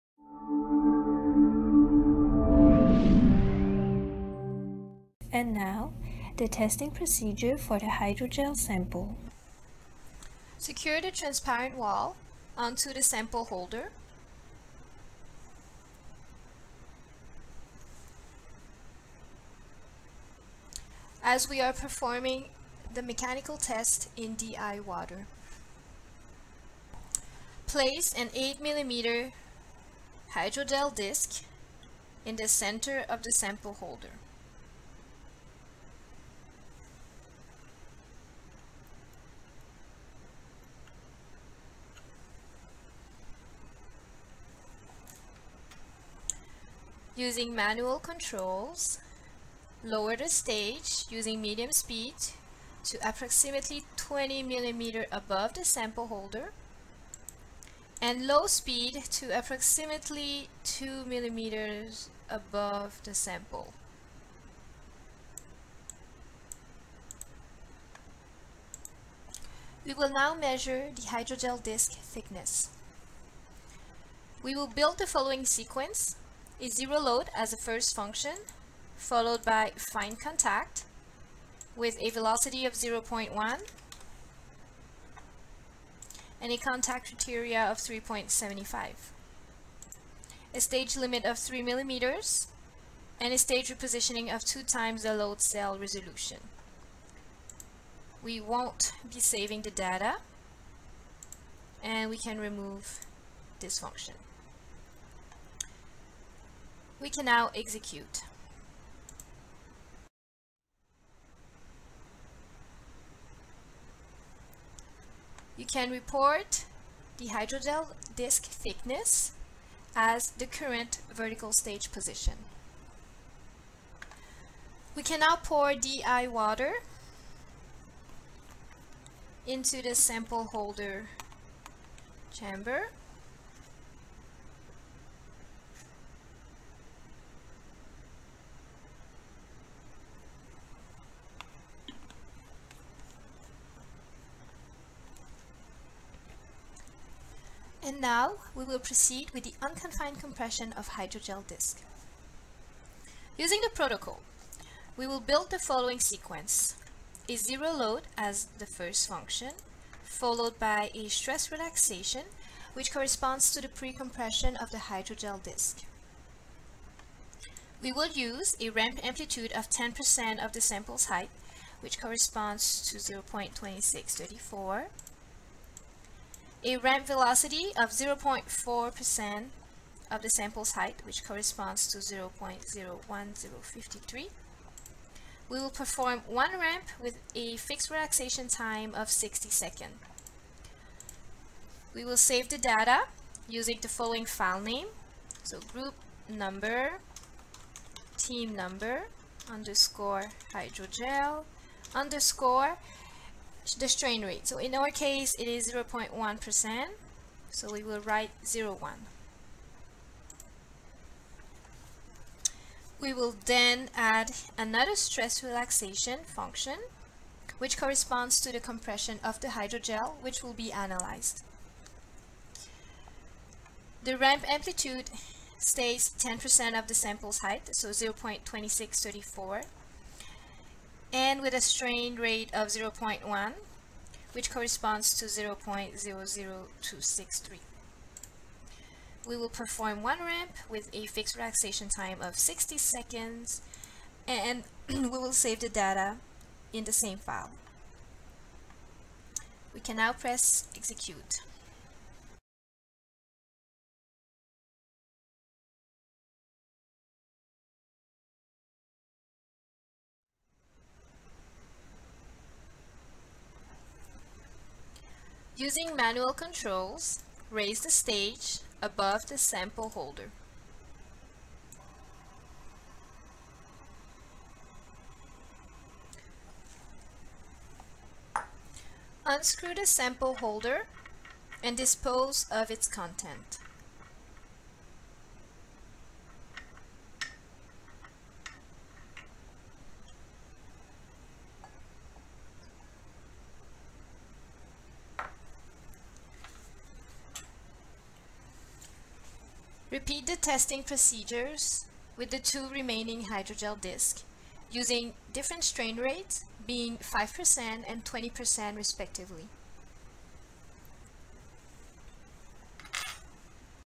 Unconfined Compression of Hydrogel Disks - Part 6 - Testing Procedure Hydrogel
The unconfined compression test is by far the most popular mechanical testing configuration to measure the mechanical properties of cylindrical samples. This undergraduate biomechanics class laboratory module consists of assessing mechanical properties of both rubber and hydrogel samples. Throughout this laboratory, each team will: prepare the rubber and hydrogel samples; test sample in unconfined compression with various stain rates; obtain the rubber and hydrogel samples' stiffness and Young's modulus for various strain rates considering the sample geometry; discuss the effect of stain rate on the Young's modulus; conclude with the effect of strain rate on Young's modulus for materials with elastic vs. non-elastic behavior.